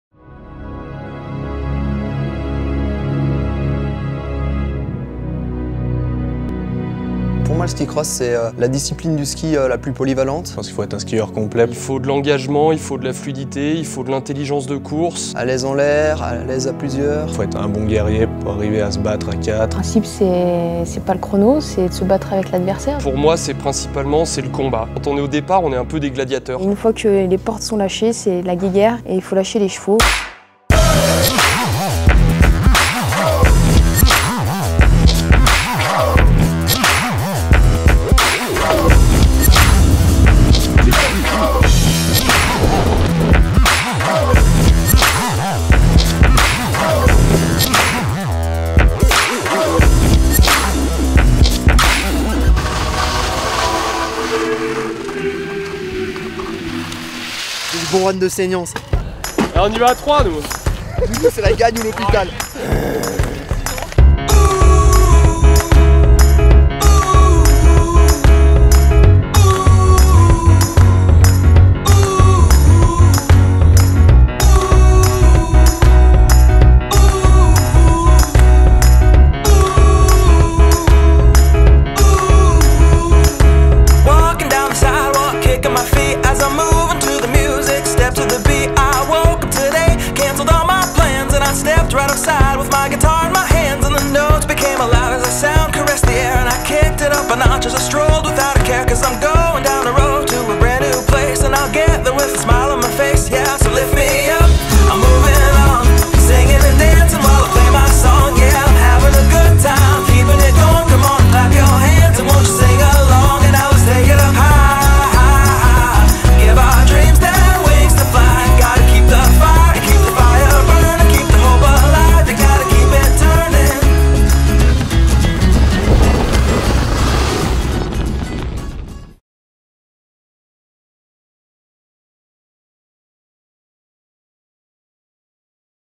DYNASTAR skis | Ski cross Team
Dynastar ski cross team with Jean-Frédérique Chapuis and Arnaud Bovolenta, Olympic Champion and Vice-Champion at Sochi.